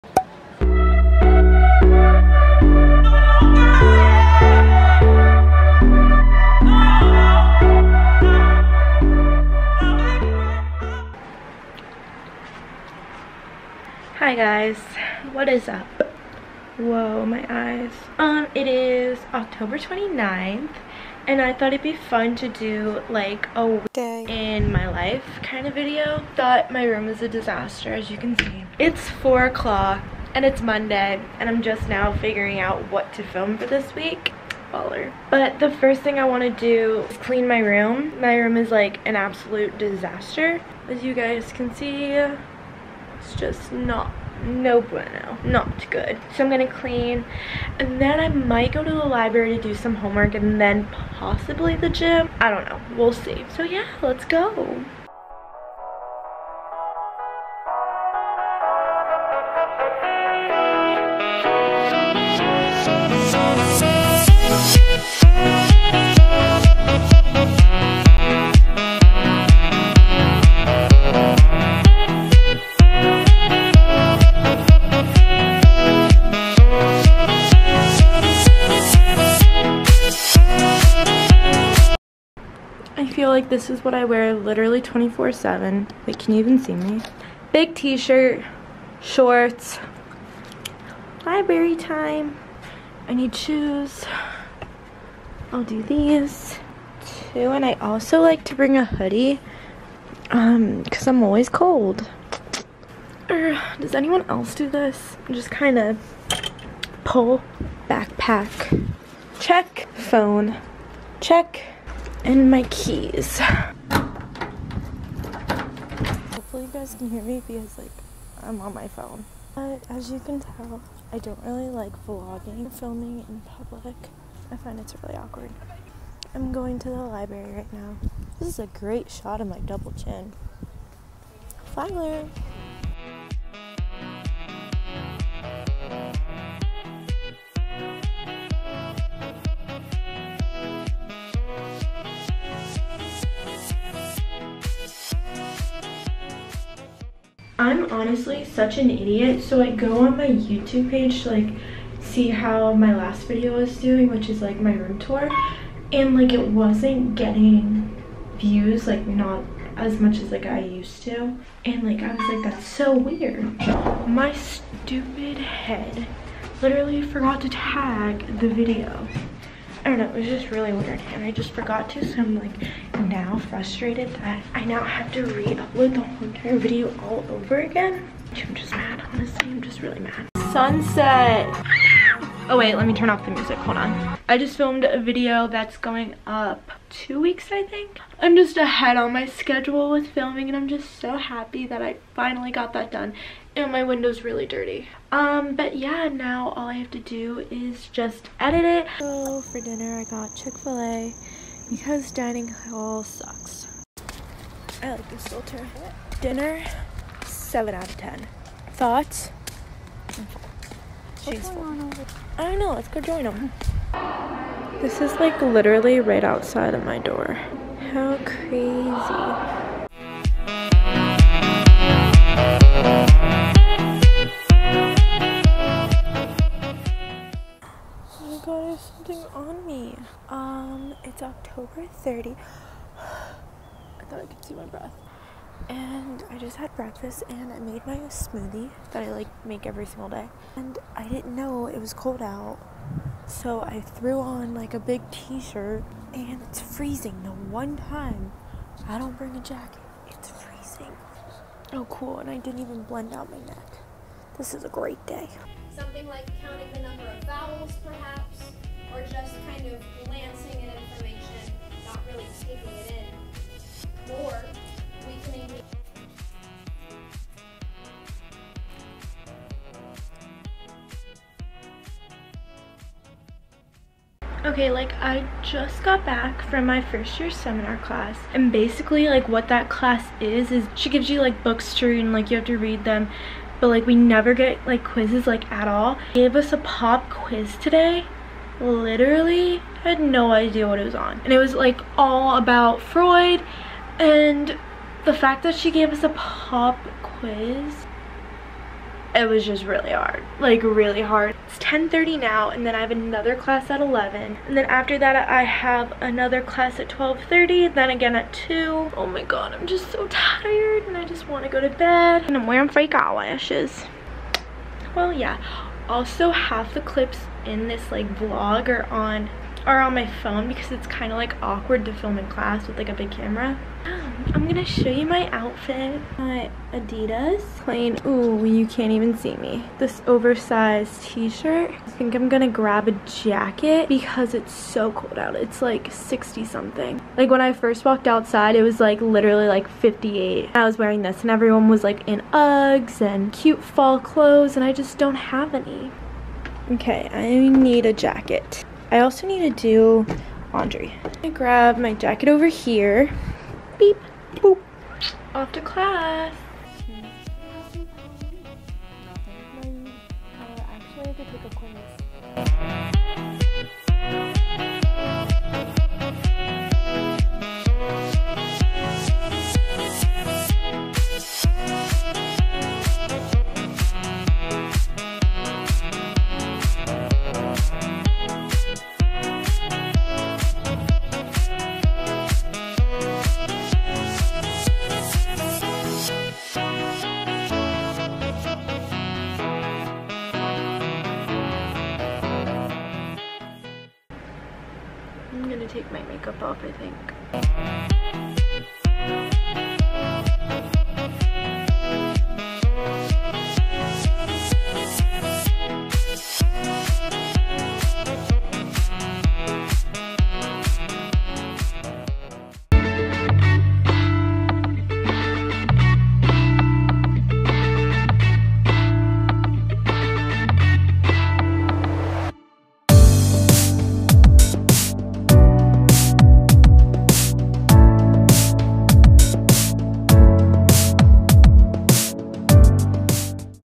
Day in My Life at Flagler College | Natalie Nicole
Hey y'all, come along with me where I go to class and clean. just a typical day for me!